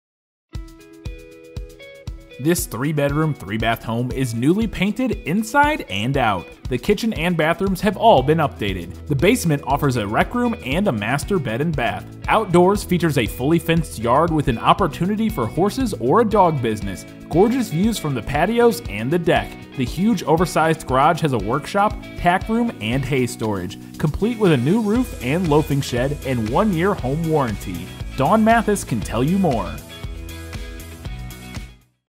Dawn Mathis presents 3930 Labrador Lane Wellington, CO | ColdwellBankerHomes.com
3930 Labrador Lane, Wellington, CO

A super opportunity for Horses, a Dog Business, a Bed and Breakfast, VRBO, Wedding Venue and more! Endless opportunities on this beautifully maintained property. The home is newly painted inside and out! Kitchen and bathrooms are updated! Outdoor features include stunning private areas with a stream/w